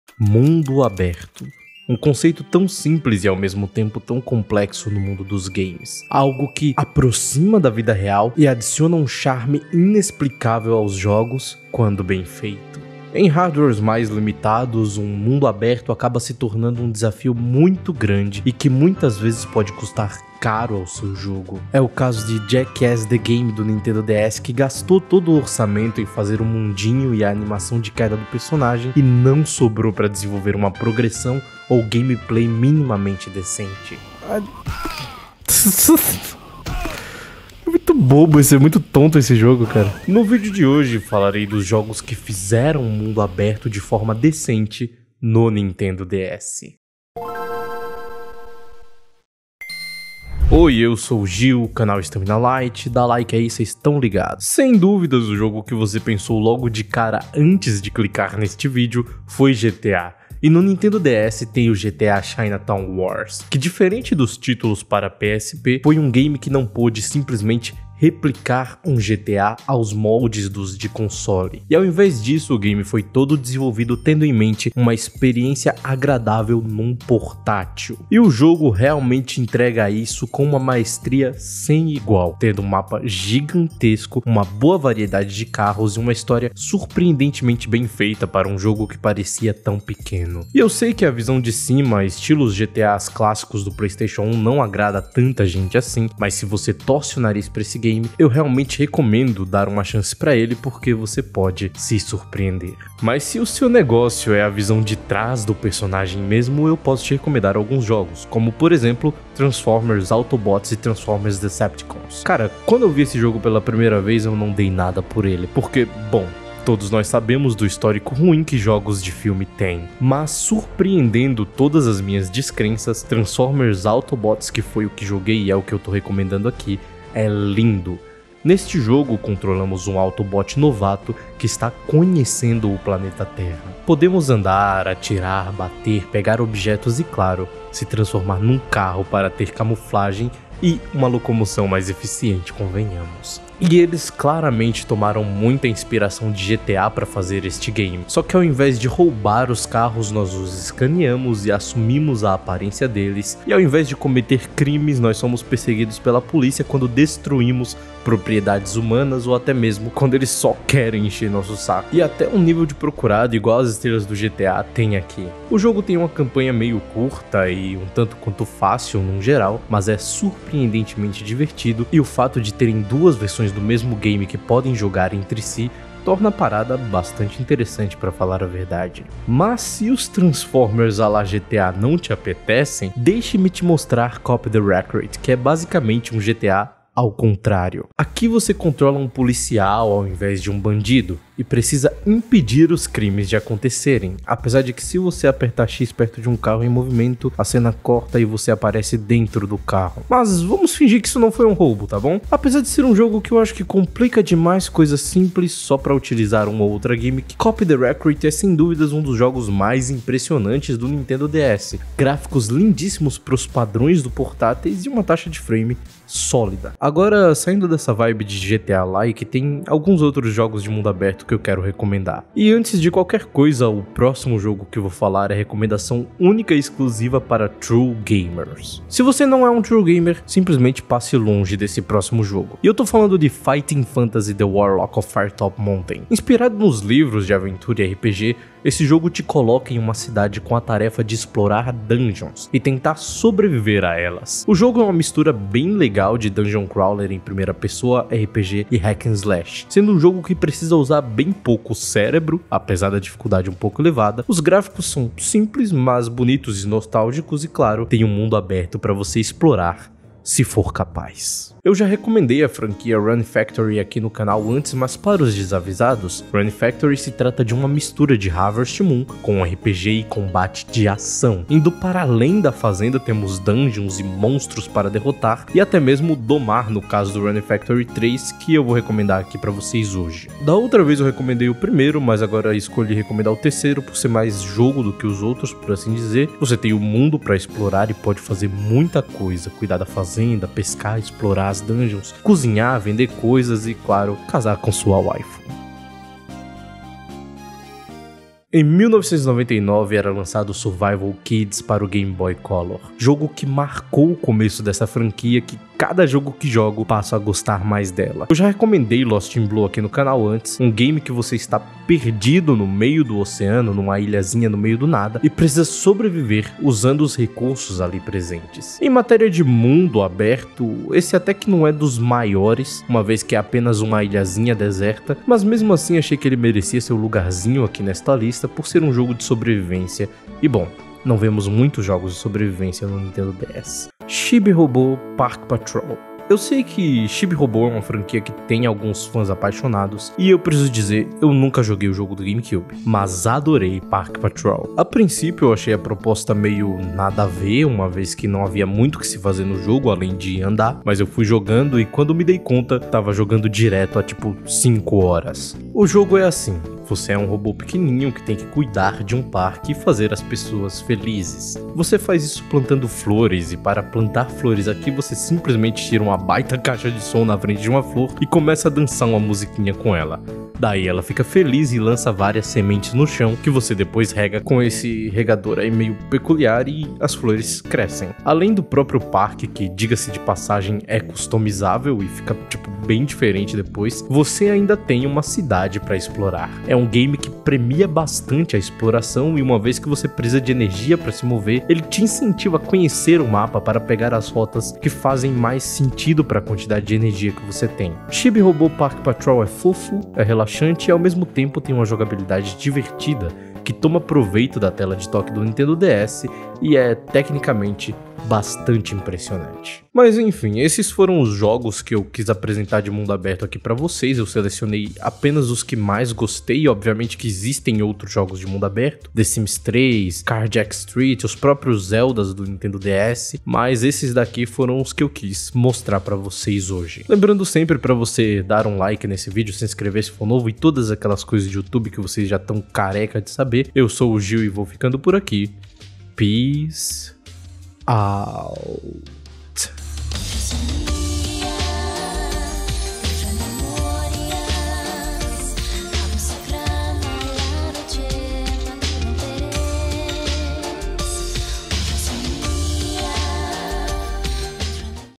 7 Jogos de MUNDO ABERTO para o NINTENDO DS! - Estamina Lite.
7 Jogos de MUNDO ABERTO para o NINTENDO DS!

Mundo aberto.
Um conceito tão simples e ao mesmo tempo tão complexo no mundo dos games.
Algo que aproxima da vida real e adiciona um charme inexplicável aos jogos quando bem feito.
Em hardwares mais limitados, um mundo aberto acaba se tornando um desafio muito grande e que muitas vezes pode custar caro ao seu jogo. É o caso de Jackass: The Game do Nintendo DS que gastou todo o orçamento em fazer o mundinho e a animação de queda do personagem e não sobrou pra desenvolver uma progressão ou gameplay minimamente interessante. No vídeo de hoje falarei dos jogos que fizeram um mundo aberto de forma decente no Nintendo DS.

Peace out C: